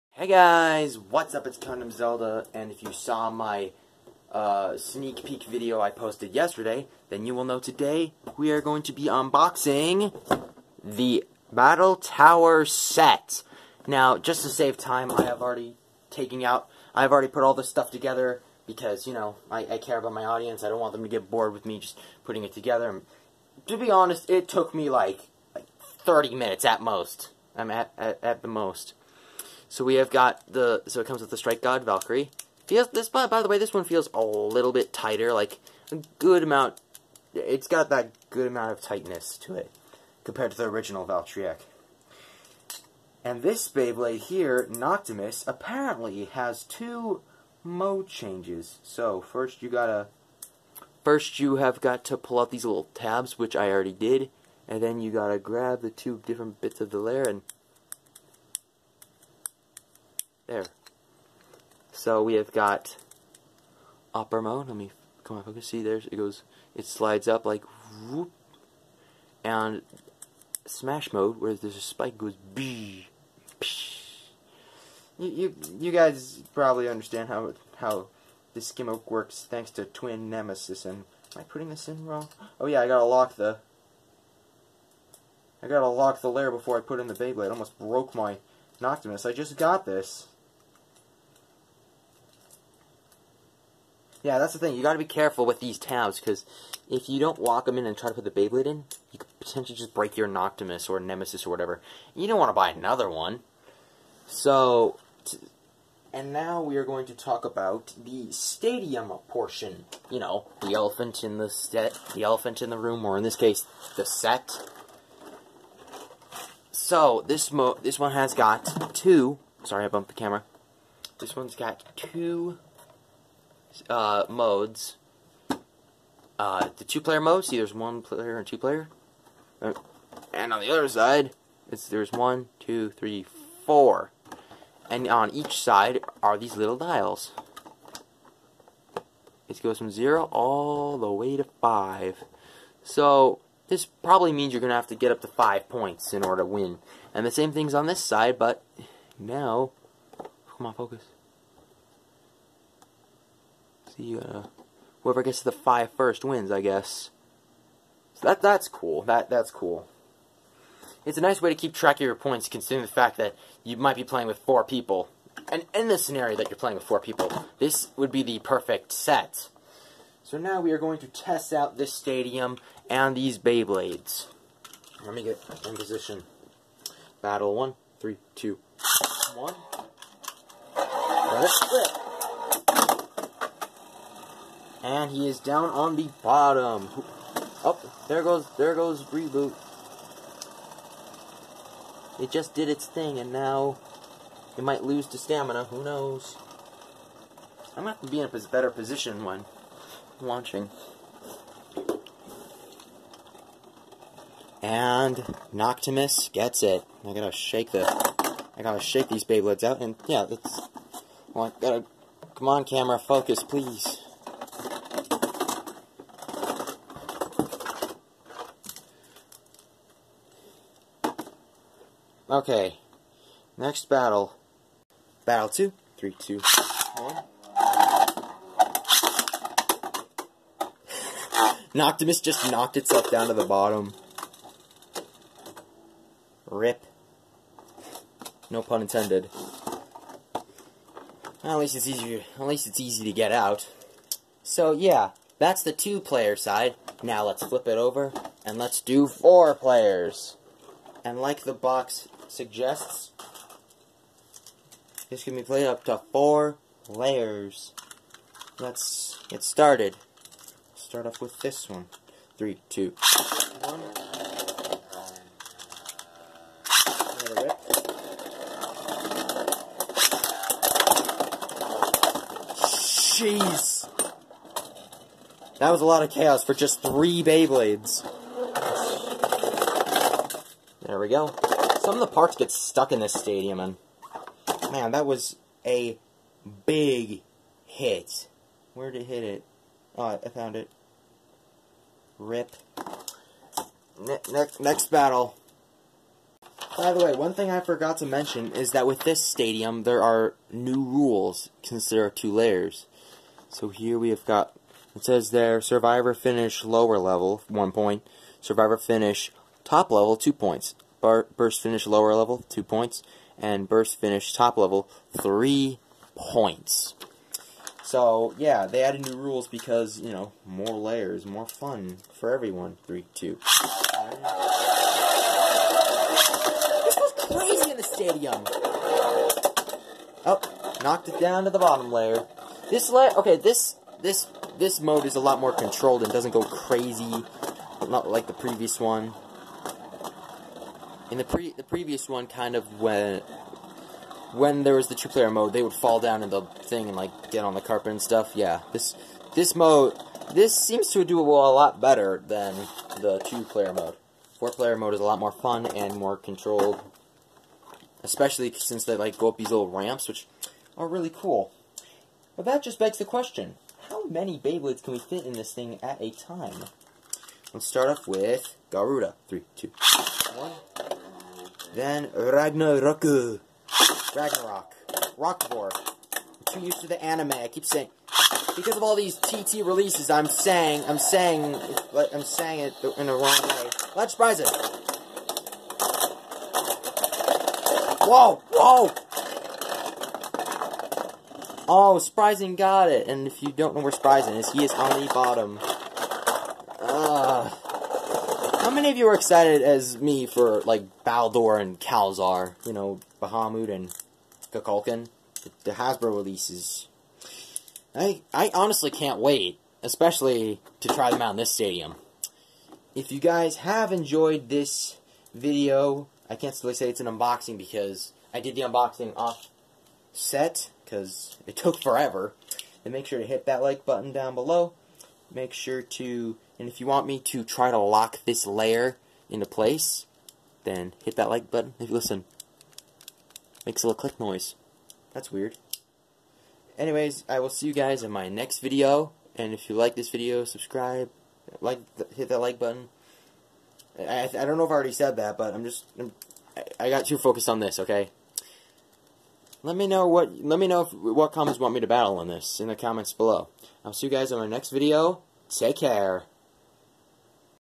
Battle tower set review plus an extra level?
In this video I review the battle tower set... Plus I discovered with the addition of the Avatar Attack Battle Set, that the stadium can have not one, not two, but three levels?!